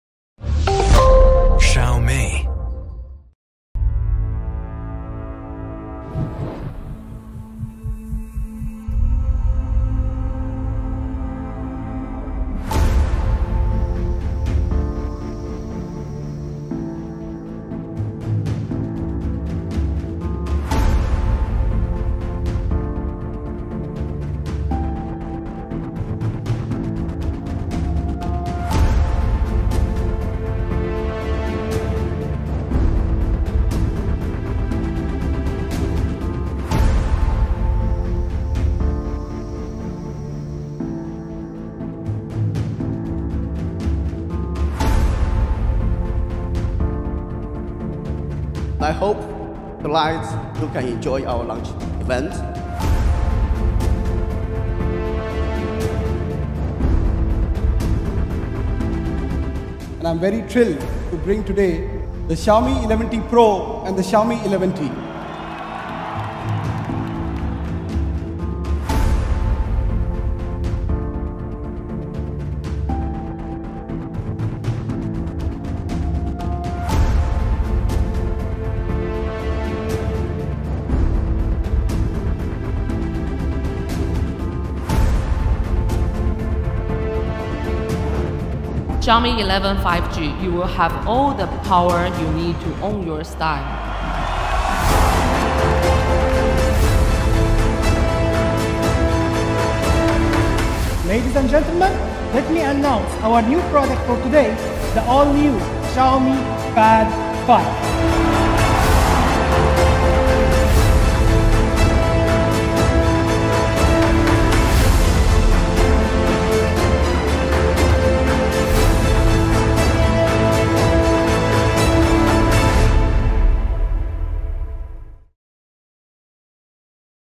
Xiaomi 11T Series Middle East launch event highlights
Missed our launch event? Here are the highlights of the most Cinemagic night of the year, featuring our management and the amazing Khalid Al Ameri!